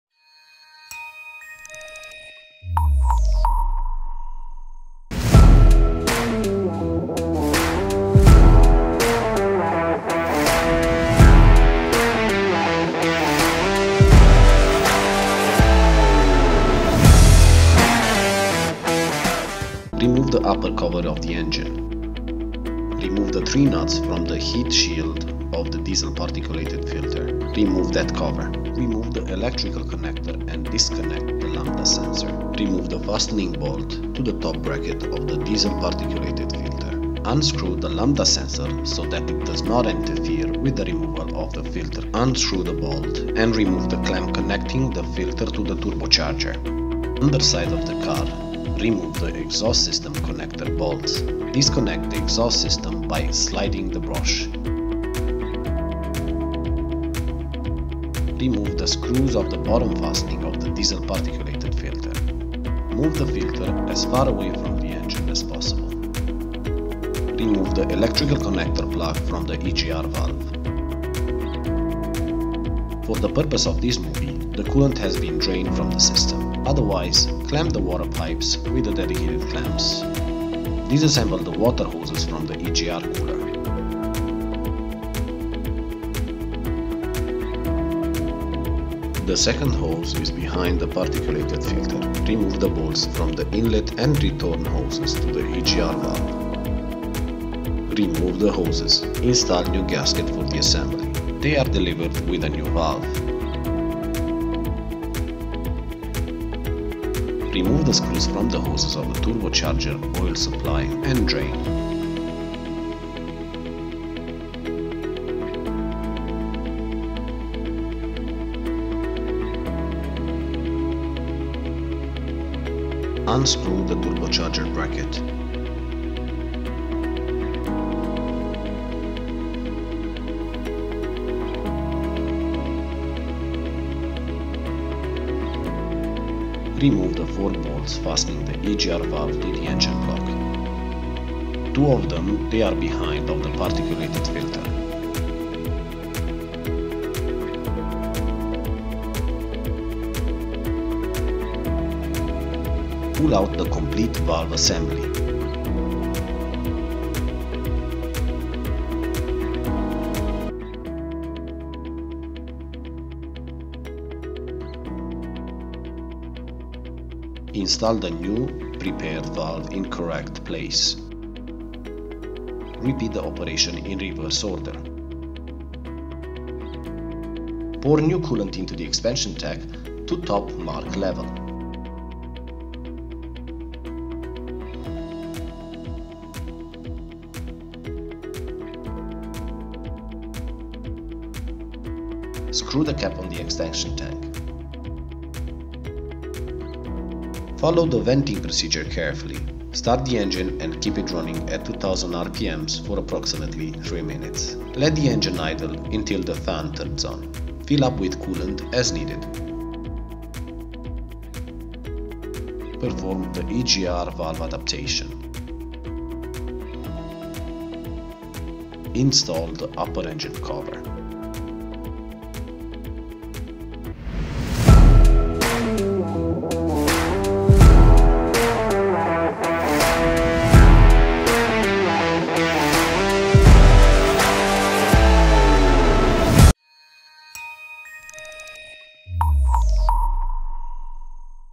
How to replace an EGR cooler on a VW 2.0 TDI engine
In this video out TECHsupport Team is presenting how to change the EGR valve on our nrforangehornet, a Volkswagen Scirocco, 2.0 TDI engine